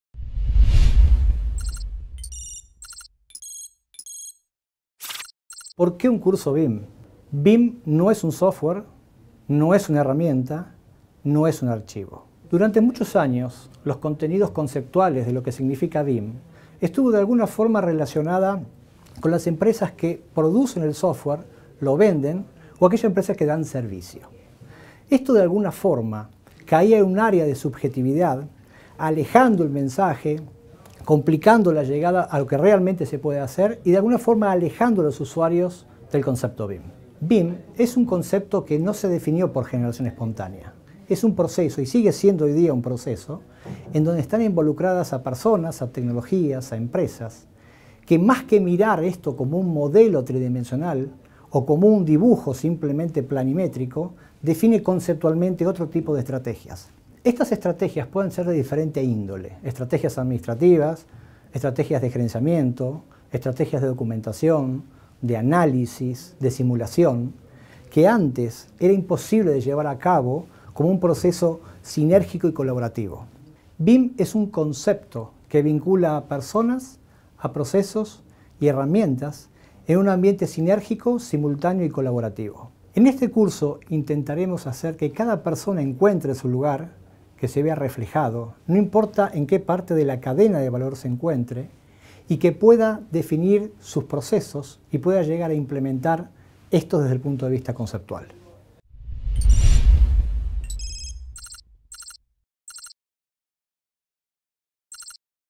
1 - Introducción al concepto BIM
A cargo del arquitecto Hector Miller, especialista en implementación BIM con más de 30 años de trayectoria en la disciplina, el curso tiene como finalidad acercar a los profesionales los beneficios del concepto BIM (Building Information Modeling). A lo largo de 5 entregas semanales, los lectores podrán acceder a los contenidos en video a través del sitio de ARQ (arq.clarin.com) y de nuestro canal de Youtube de forma gratuita, sin inscripción y en cualquier momento.

Auspica Knauf
Desarrolla Clarín Arquitectura + Miller&Co